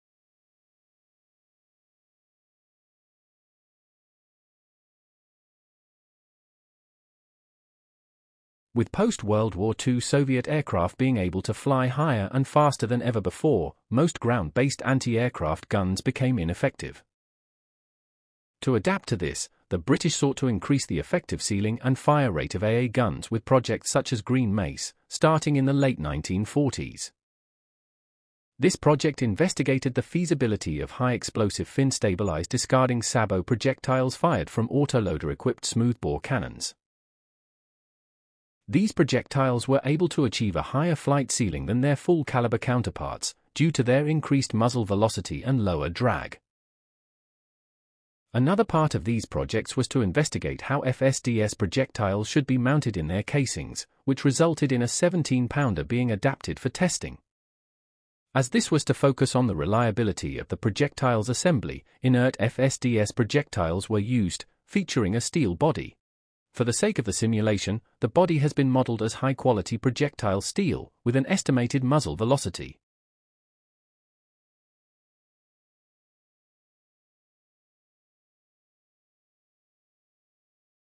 1940's APFSDS for the 17pdr vs PANTHER II + History of British FSDS | Armour Penetration Simulation
The simulation presents the an experimental Fin-Stabilised Discarding-Sabot (FSDS) round for the 17pdr impacting the upper glacis of the Panther II.

After WWII, the British experimented with smoothbore cannons and High-Explosive FSDS (HEFSDS) projectiles in order to improve the effectiveness of ground based Anti-Air systems. This followed on from similar projects undertaken by the Germans in WWII, who also developed HEFSDS projectiles, mainly to increase the range of artillery. One of the areas investigated by the British was how an FSDS projectile should be mounted in its casing, which resulted in a 17pdr being adapted for testing. The test projectiles were inert, having a steel body and likely an aluminium tail. While these were never intended for an anti-tank role, the simulation has assumed they were made of projectile grade steel and fired at an estimated muzzle velocity. The velocity estimated gives the projectile+sabot less kinetic energy than a standard 17pdr AP or APDS round, so it is potentially conservative. However, it is likely slightly lower because a small portion of the casing would have been occupied by the FSDS mounting assembly instead of propellant.

The Panther II was to feature numerous upgrades, including the armour, turret design, main gun, night vision, engine, and component standardisation with the Tiger II. It however never entered service and only prototypes were produced. The original Panther's 80mm upper glacis was vulnerable to 122mm and 100mm cannons, resulting in the proposed 100mm upgrade. The 100mm upper glacis has been modelled as good quality 250BHN Rolled Homogenous Armour (RHA).

Amazing Thumbnail Artwork From: Warthunder